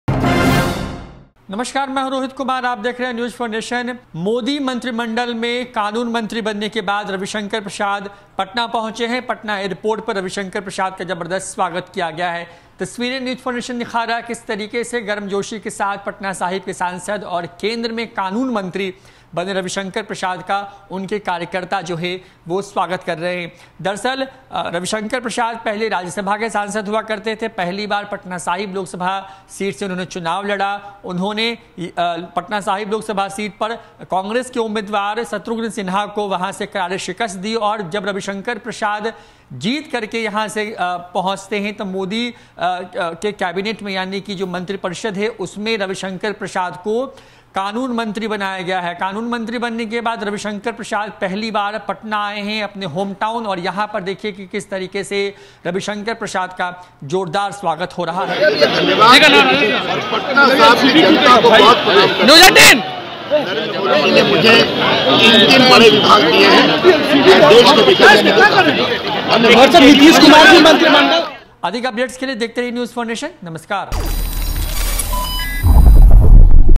Ravi Shankar Prasad Modi Cabinet में Minister बनने के बाद पहली बार आये पटना जबरदस्त स्वागत
Ravi Shankar Prasad  Modi Cabinet में Minister बनने के बाद पहली बार आये पटना जबरदस्त स्वागत | News4Nation